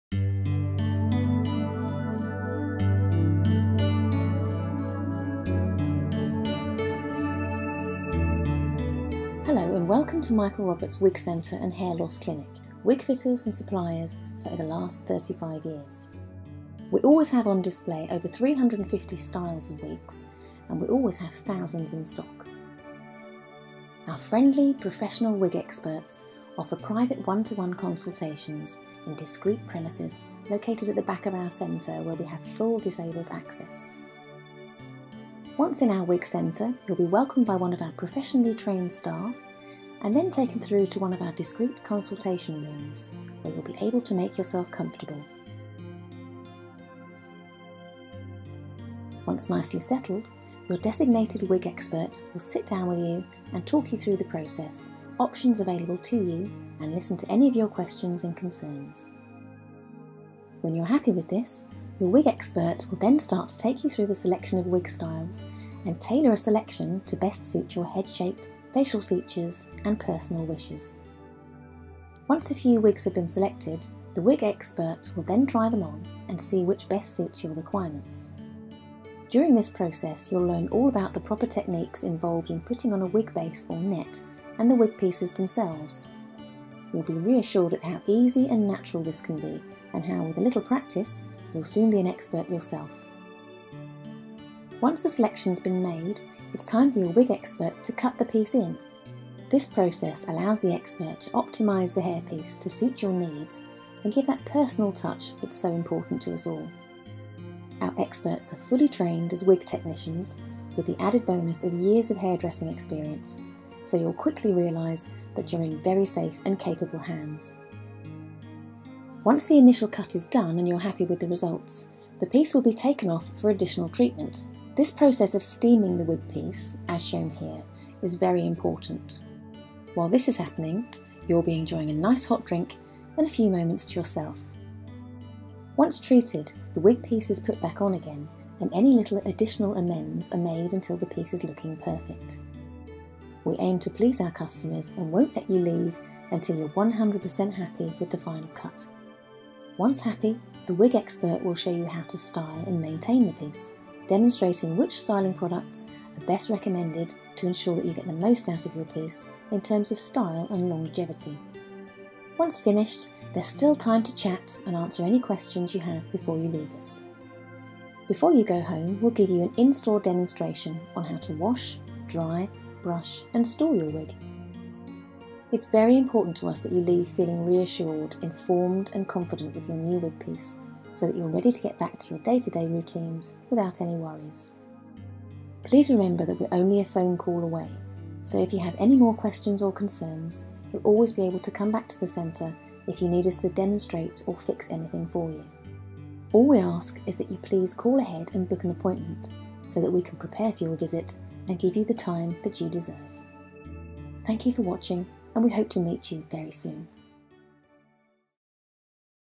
MICHAEL ROBERTS WIG CENTRE & HAIR LOSS CLINIC - WELCOME
Welcome to Michael Roberts Wig Centre & Hair Loss Clinic. This welcoming video will start by introducing you to our excellent facilities and our friendly and fully trained wig experts. Let us guide you through the process of choosing, cutting and styling your new wig piece. Once you have watched this video, you will hopefully feel a lot more confident in choosing Michael Roberts. Thank you.